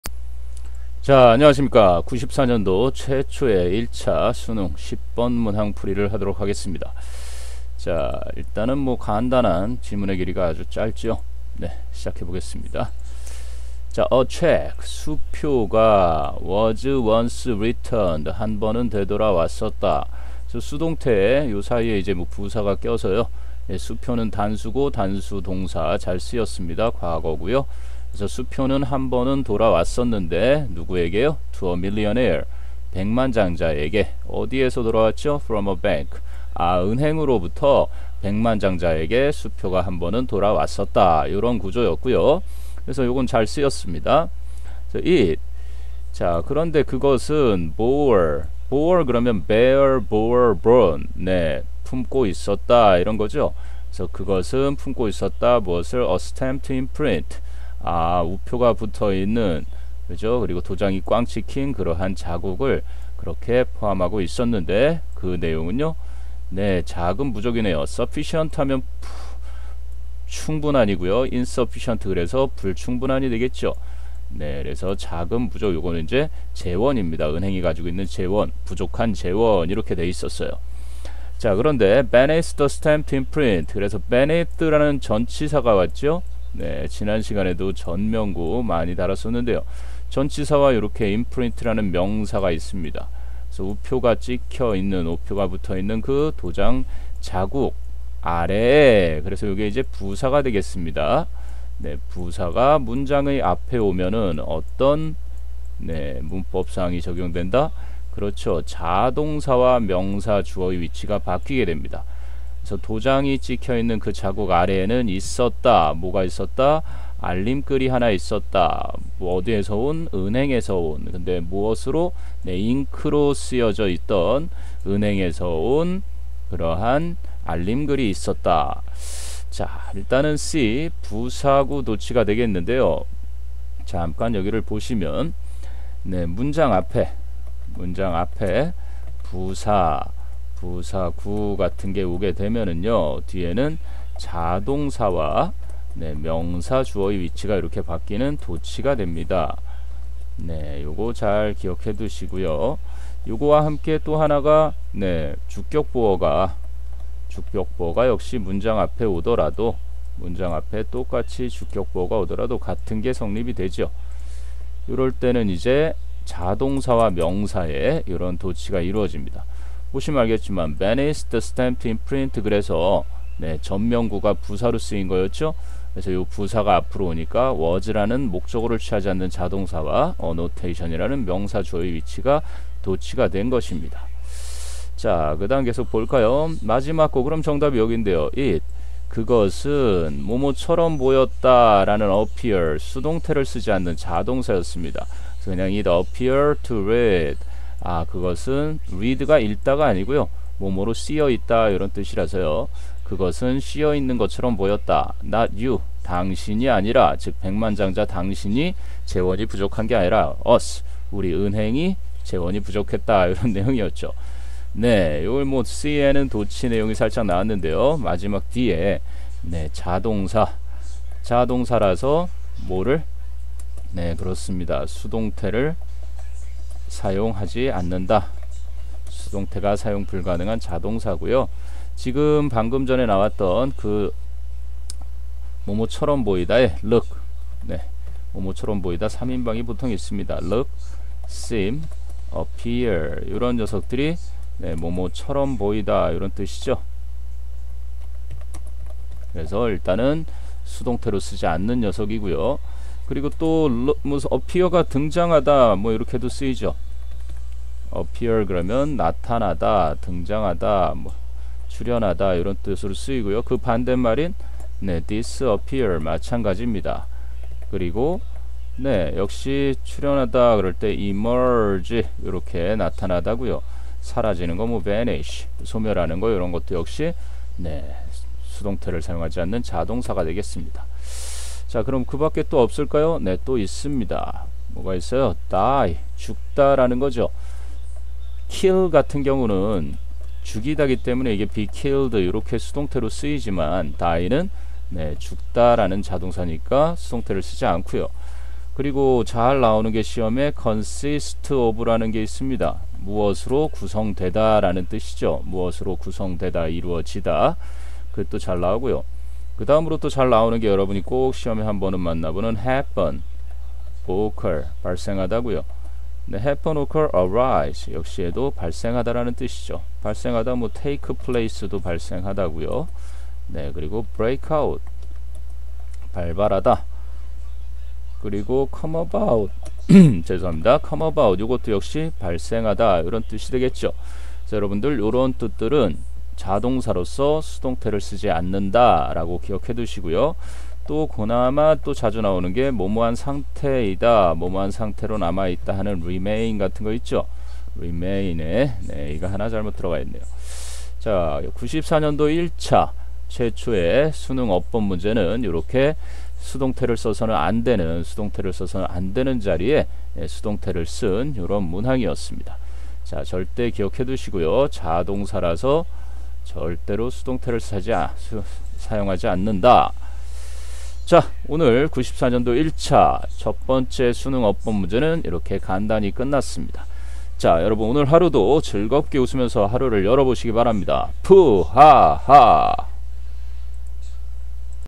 수능기출 어법문제 1994 1차 수능어법 10번
1994 1차 수능기출  10번 어법문제 풀이 동영상입니다.
직독직해와 함께 영어학습에 도움이 되시기를 바랍니다^^

1. 다음 글을 읽고, 밑줄 친 부분 중 어색한 부분이 있는지를 결정하고, 만약 있다면 그 부분은? 【1994학년도 1차 10번 문항】
(A)A check was once returned to a millionaire from a bank. (B)It bore a stamped imprint: "Insufficient Funds." (C)Beneath the stamped imprint was a notation from the bank in ink. (D)It was appeared to read, "Not you ― us!"

①(A)   ②(B)   ③(C)
④(D)   ⑤어색한 부분 없음